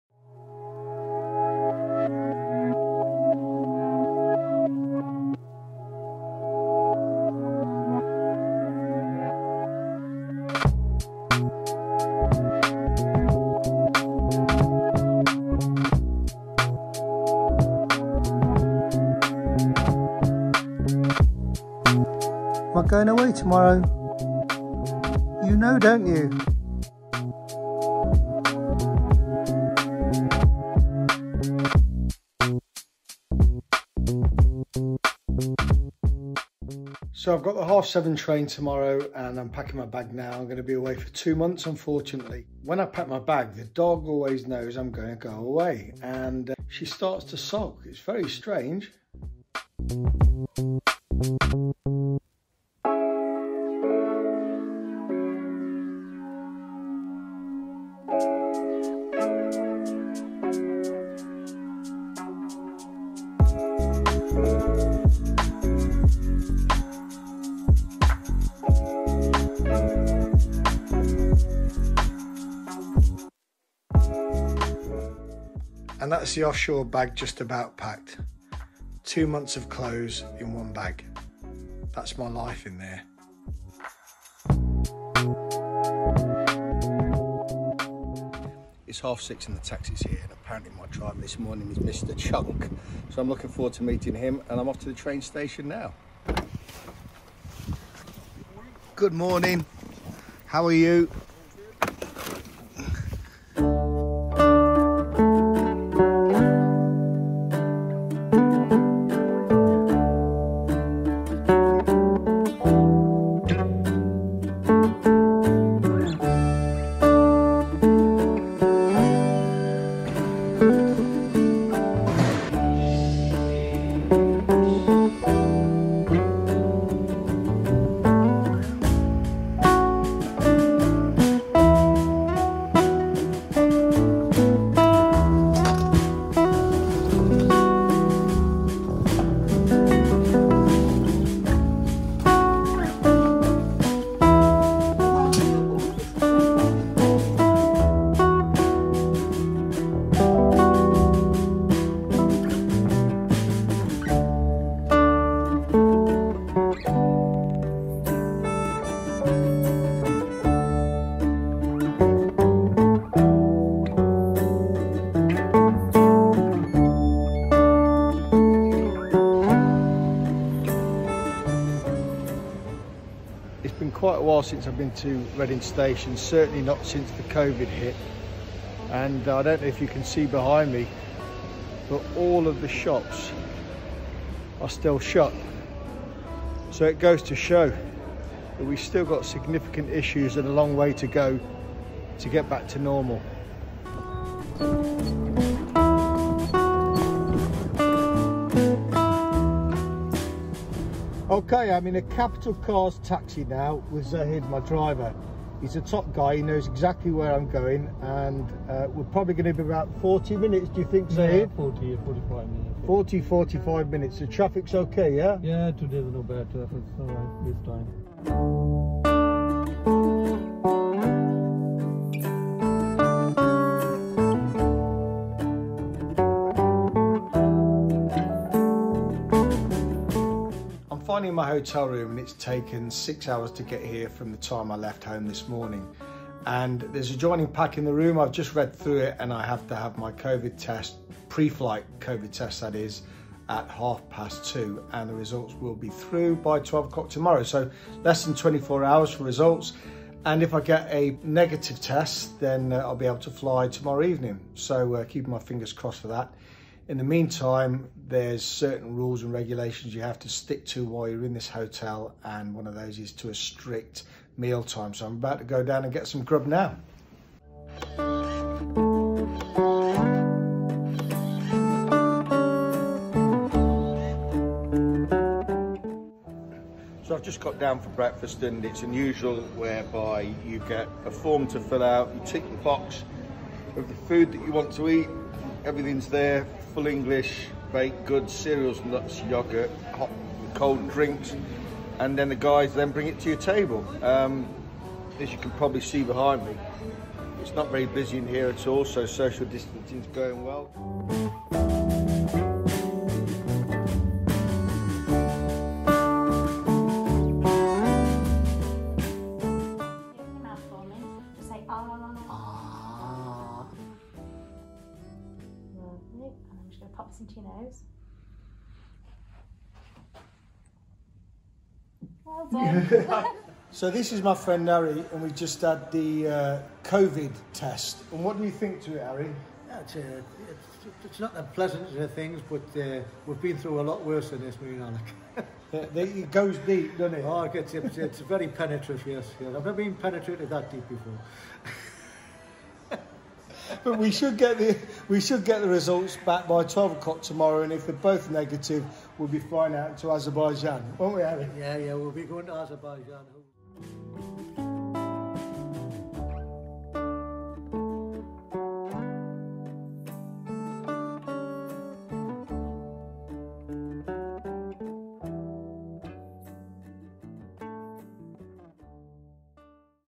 COVID 19 Fit to Fly Certificate - Getting Tested at Heathrow Airport
This upload explains more about the COVID 19 Fit to Fly certificate and getting tested at Heathrow to fly safe. It's needed to board flights from UK Airports. Why not take the fly safe COVID test at Heathrow Airport to speed things up?

Results of the COVID test were emailed to us within 24 hours of flight departure and the COVID certificate for travel was accepted at all airports on the transit route without any problems.

The video also shows how quiet the UK passenger transport sector is at present with lots of shops closed and empty seats due to the coronavirus pandemic.

Taxi company in Plymouth was Need-A-Cab. Taxi company in Reading was Capital Cards. They are both great companies and I've used them for years without issue.

Mi viaje al aeropuerto de Londres Heathrow para obtener un certificado COVID fit to fly estuvo ocupado. Pero es necesario para abordar vuelos desde aeropuertos del Reino Unido. ¿Por qué no realizar la prueba COVID fly safe en el aeropuerto de Heathrow para acelerar las cosas?

Los resultados de la prueba COVID nos fueron enviados por correo electrónico dentro de las 24 horas posteriores a la salida del vuelo y el certificado COVID para viajar fue aceptado en todos los aeropuertos de la ruta de tránsito sin ningún problema.

El video también muestra cuán silencioso está el sector del transporte de pasajeros del Reino Unido en la actualidad con muchas tiendas cerradas y asientos vacíos debido a la pandemia del coronavirus.

La compañía de taxis en Plymouth era Need-A-Cab. La compañía de taxis en Reading era Capital Cards. Ambas son grandes empresas y las he usado durante años sin problemas.